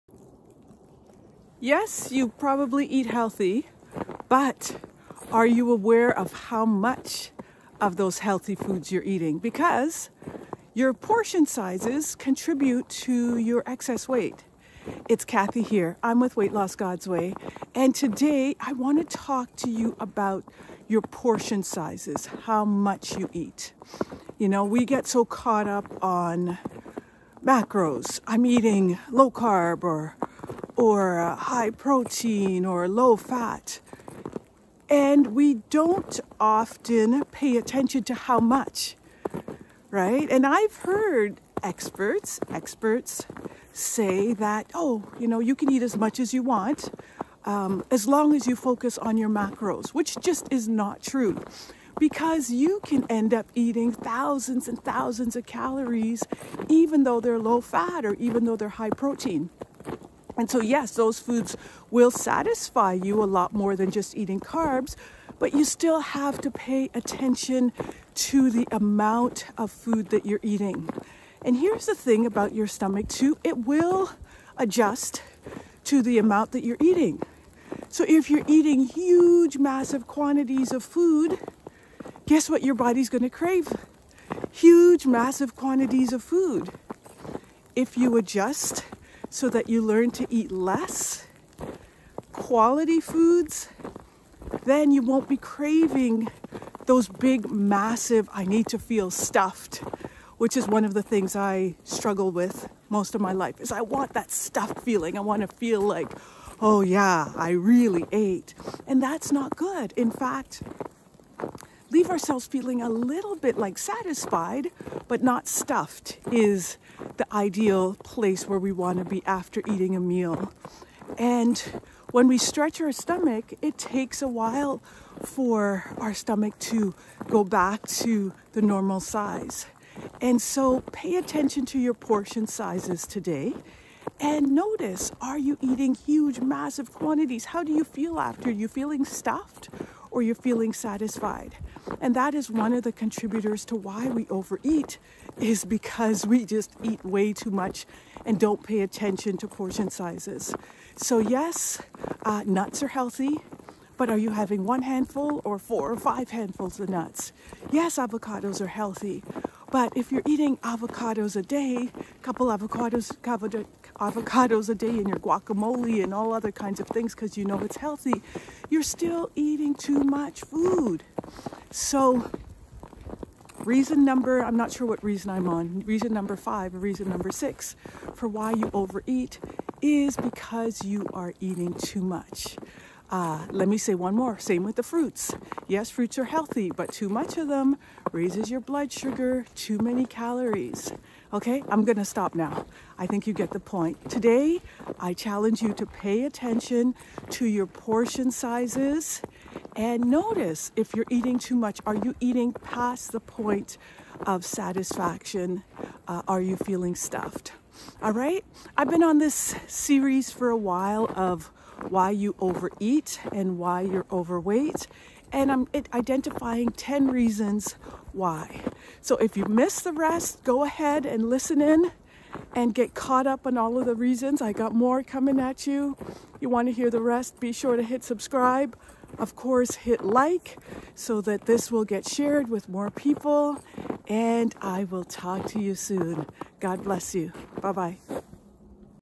10 Reasons Why You Overeat. Reason #5- Portion Sizes
Faith- Centred Weight Loss Expert and Author, Cathy Morenzie talks about how your healthy portion sizes contributes  to excess weight.
Frustrated with your weight?
to learn a radically different approach to successful weight releasing.